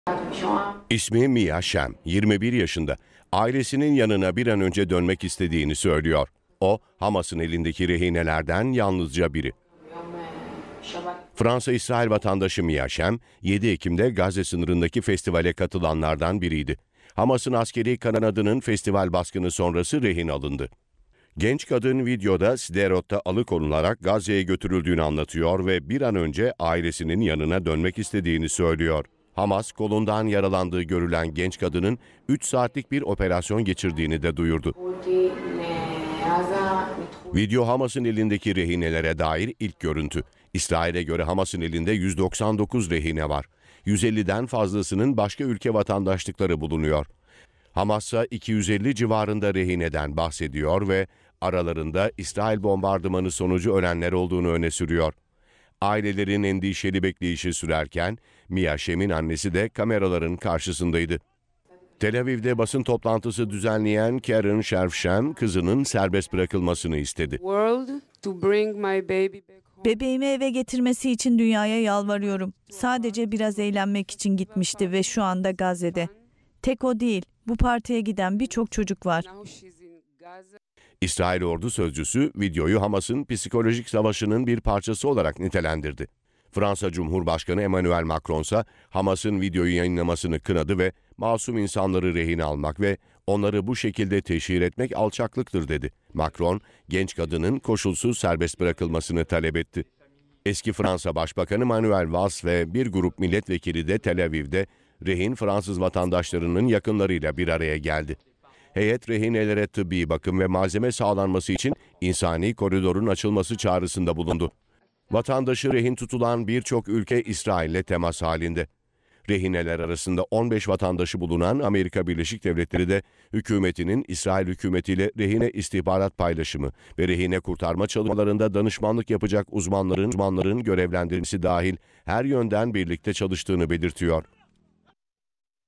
Hamas rehinelerini ilk kez gösterdi: Gazze'deki rehine sayısı kaç?
Hamas'ın elindeki rehinelerin durumu, İsrail-Gazze hattındaki çatışmalarda büyük bir endişe oluşturuyor. Hamas'ın askeri kanadı, İsrailli bir rehinenin görüntülerini yayınladı. Videoda Gazze'de tutulduğunu söyleyen genç kadın, serbest bırakılması için çağrıda bulunuyor. İsrail, videoyu Hamas'ın psikolojik savaşının bir parçası olarak nitelendirirken, ailelerin endişeli bekleyişi sürüyor.